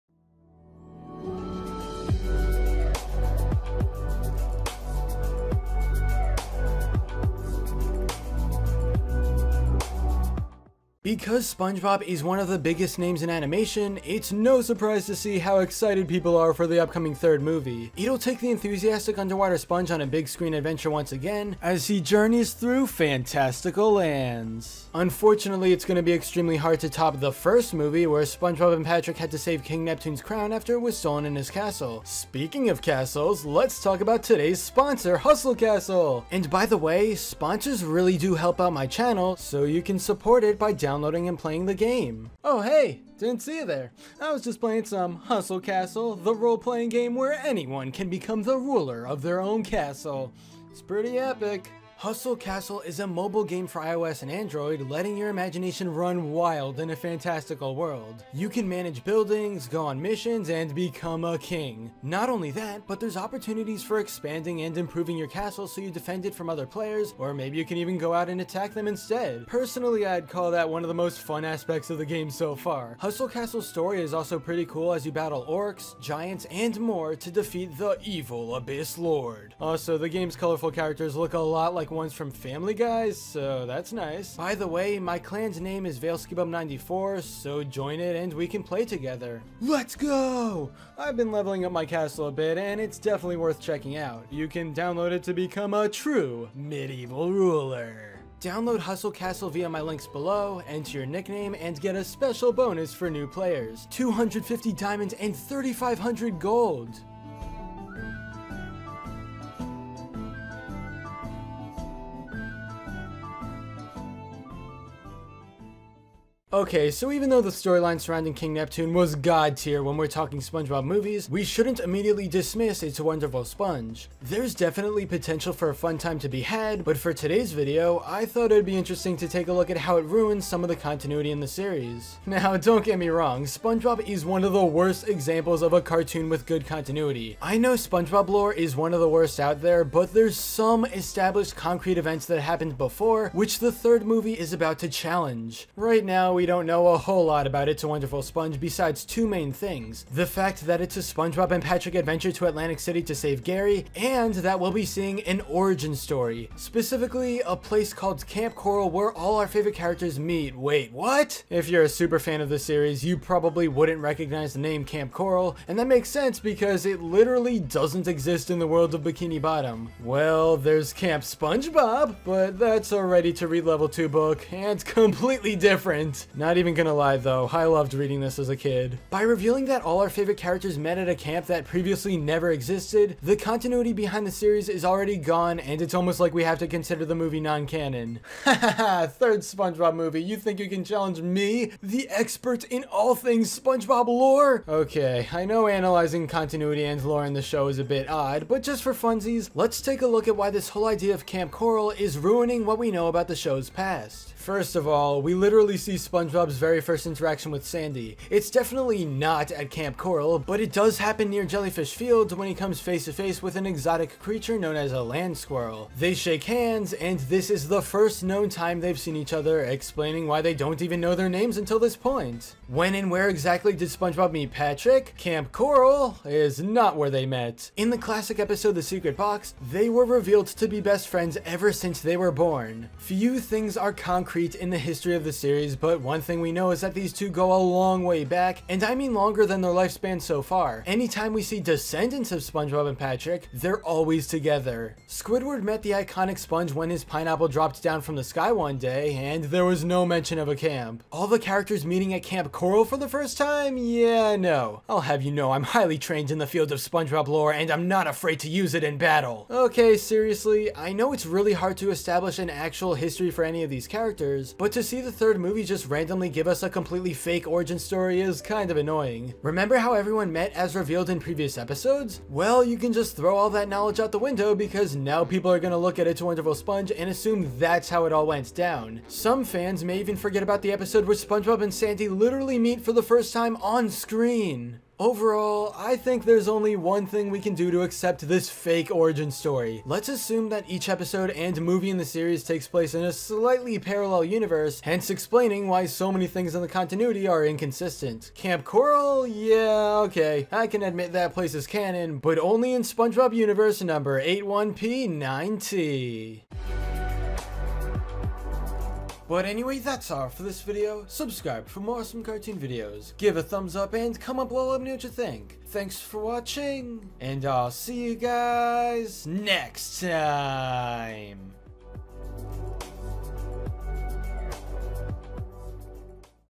Spongebob's "It's a Wonderful Sponge" Movie RUINS Series Continuity?
The brand new Spongebob movie is on the way, but it's also challenging some of the previously established events in the series... ruining the continuity!

✭ SOCIAL MEDIA ✭